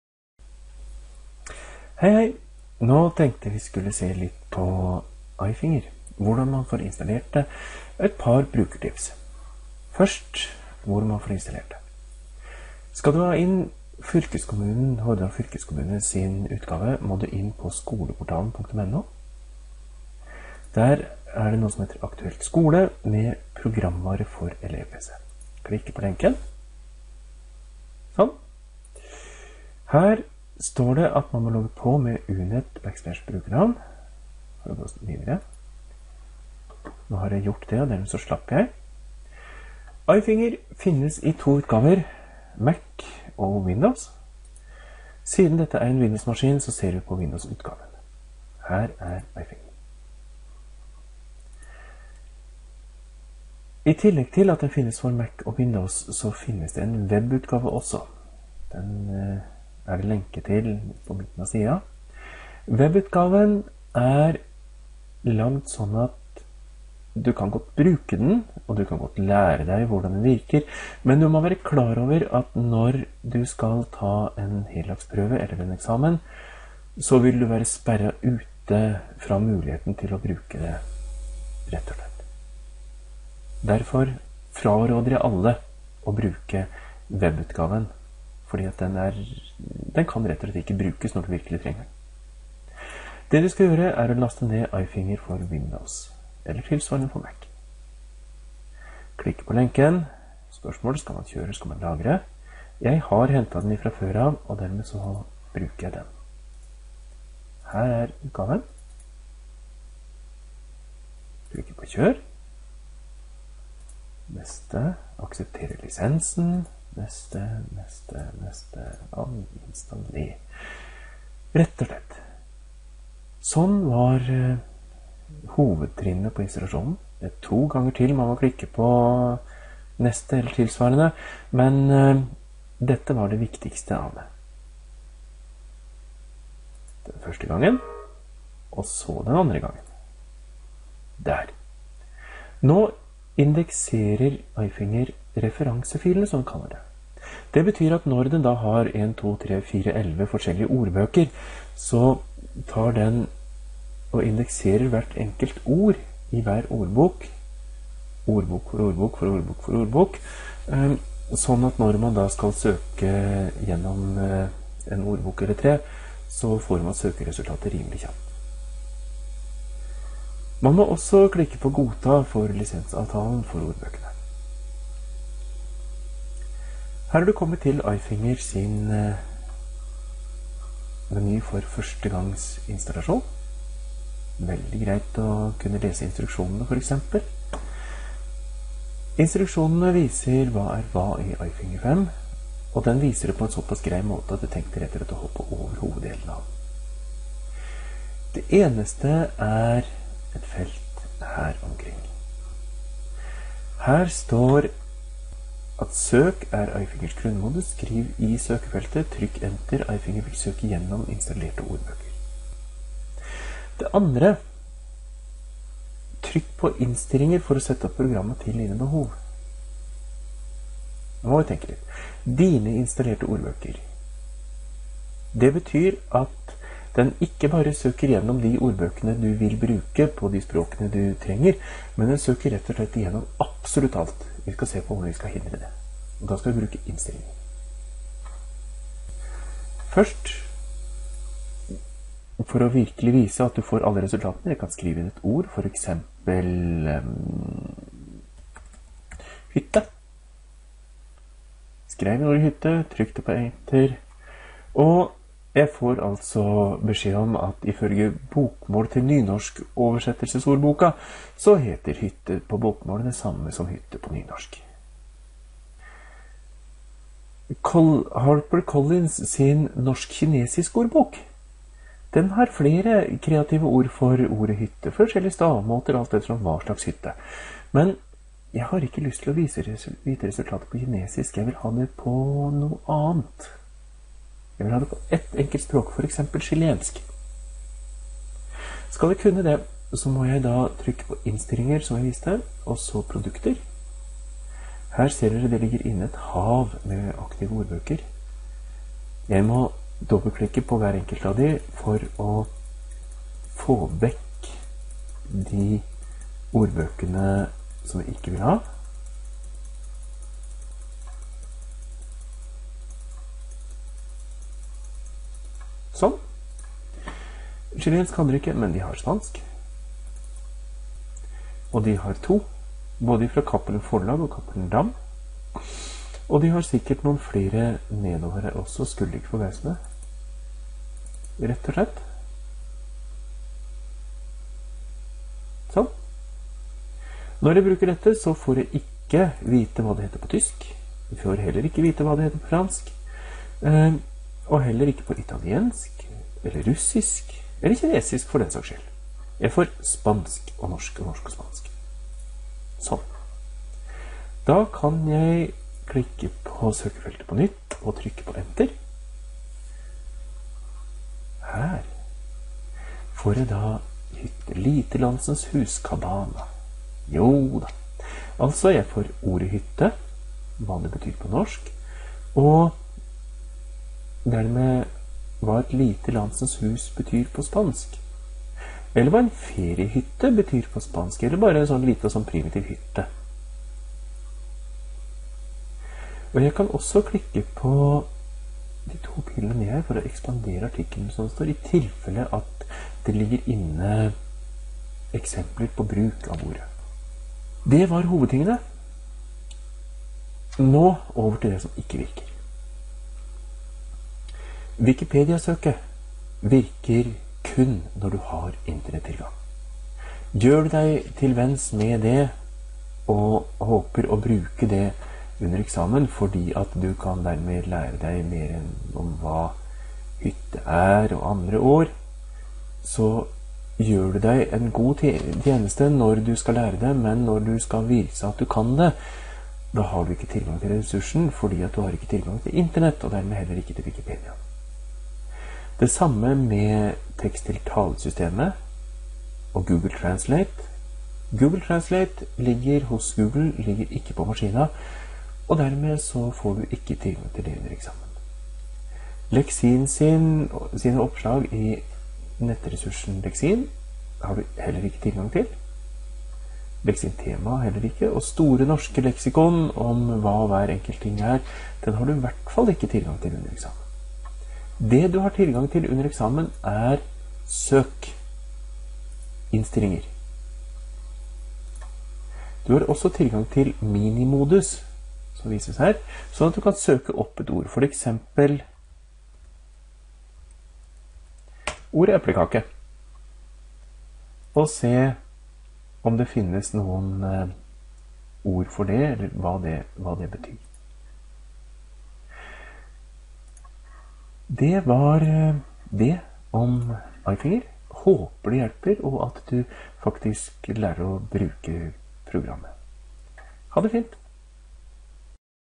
iFinger - HFK
HFK sin installasjon av iFinger - hvordan og noen tips